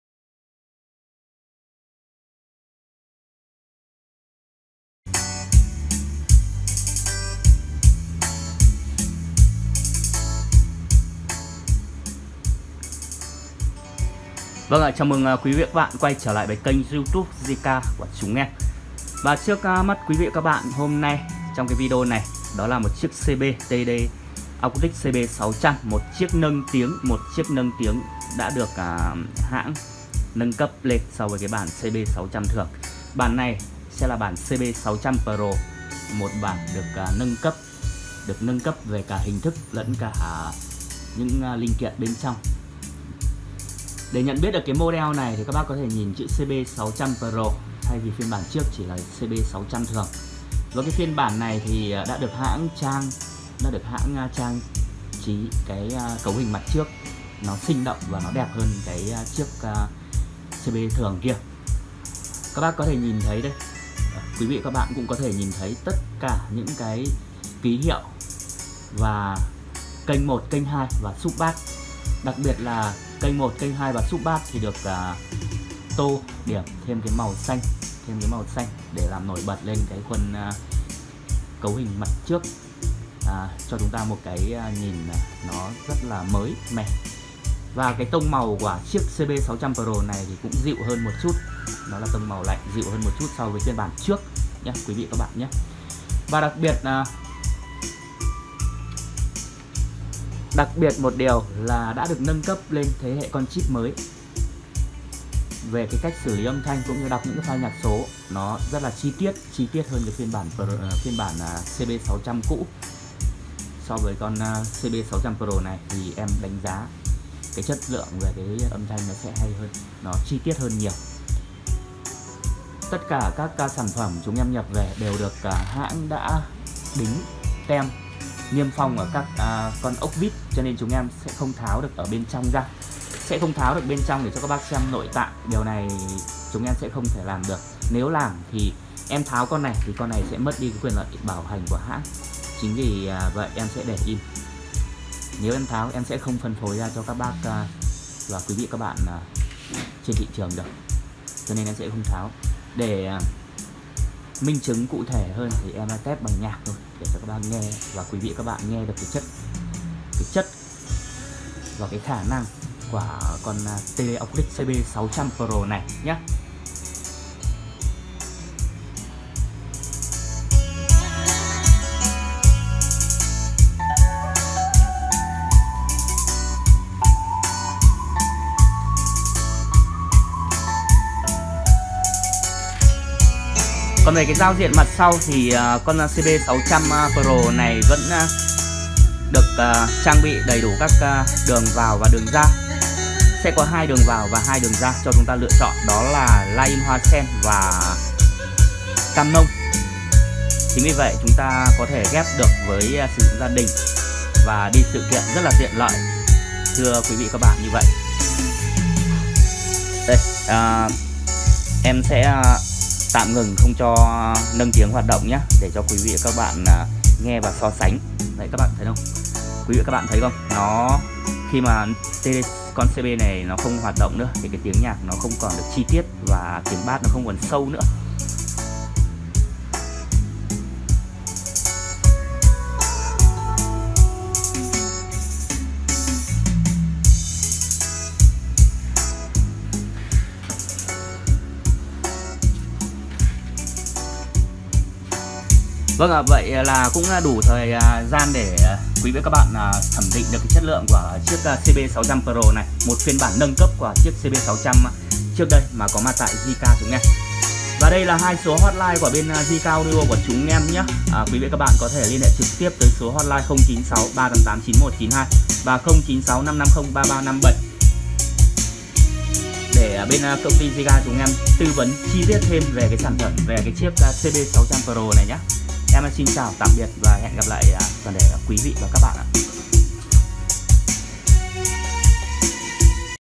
✅ĐẲNG CẤP MÁY NÂNG TIẾNG HÁT CB600 Pro CHÍNH HÃNG đã về :JK Audio 0963 88 9192 - 0965 503 357
✅Ngày hôm nay hãng TD Acoutic có thêm 1 bản nâng tiếng CB600 Pro bản này cực cao cấp, lọc sạch tạp âm và triệt tiêu dải âm thừa, nâng tông nhạc mạnh mẽ và trong trẻo